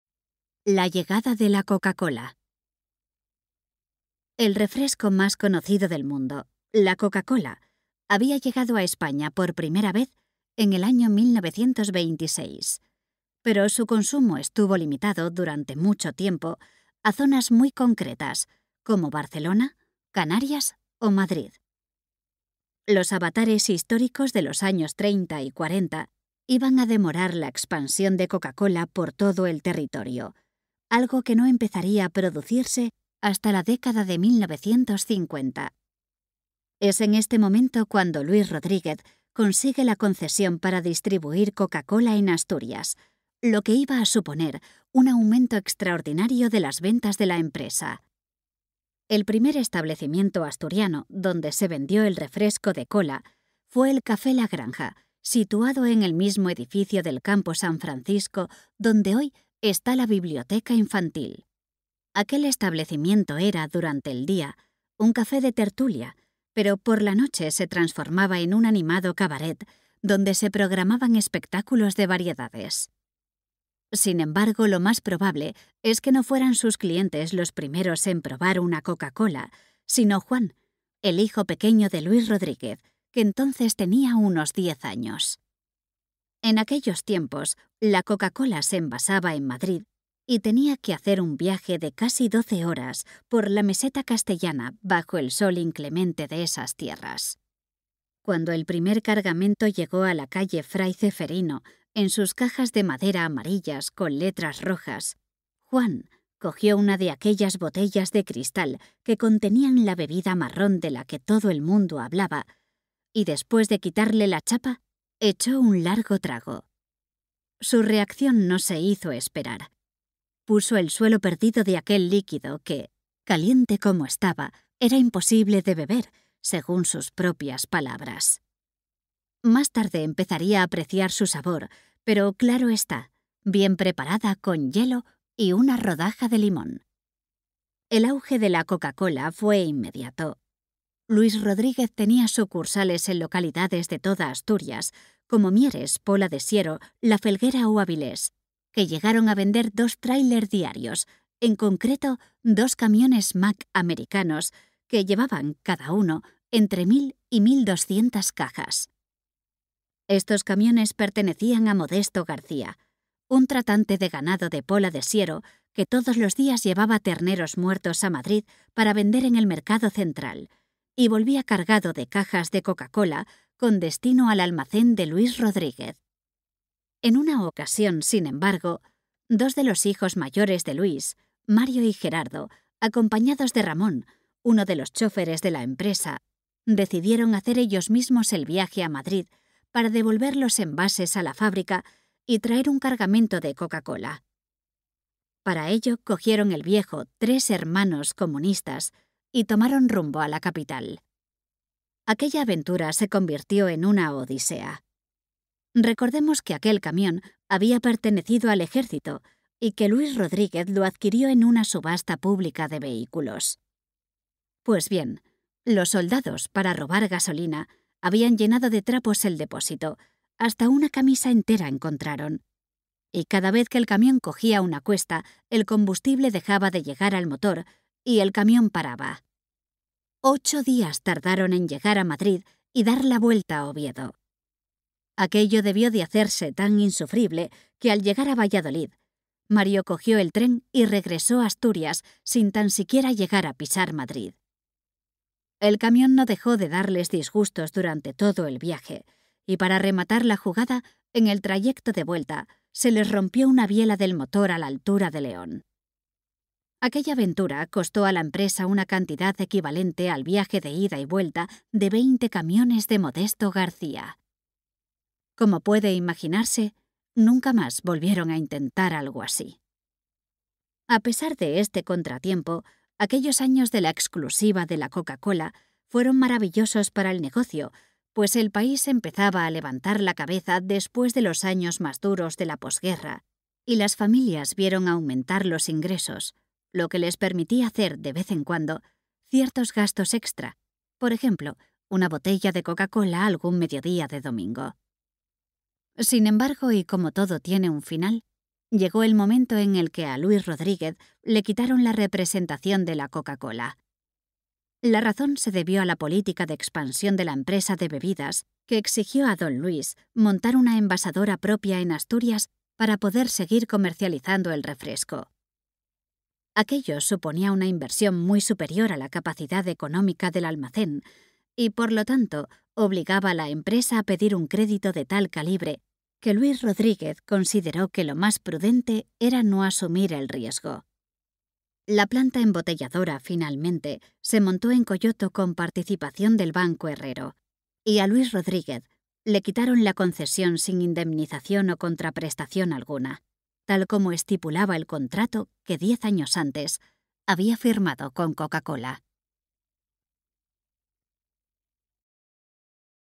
Capítulo 3: La llegada de la Coca-Cola
El refresco más conocido del mundo, la Coca-Cola, había llegado a España por primera vez en el año 1926, pero su consumo estuvo li- mitado, durante mucho tiempo, a zonas muy concretas, como Barcelona, Canarias o Madrid. Los avatares históricos de los años 30 y 40 iban a demorar la expansión de Coca-Cola por todo el territorio, algo que no empezaría a producirse hasta la década de 1950. Es en este momento cuando Luis Rodríguez consigue la concesión para distribuir Coca-Cola en Asturias, lo que iba a suponer un aumento extraordinario de las ventas de la empresa.

'Trabajo y Valores - La historia de Hijos de Luis Rodríguez' es un proyecto de masymas supermercados en Asturias y León.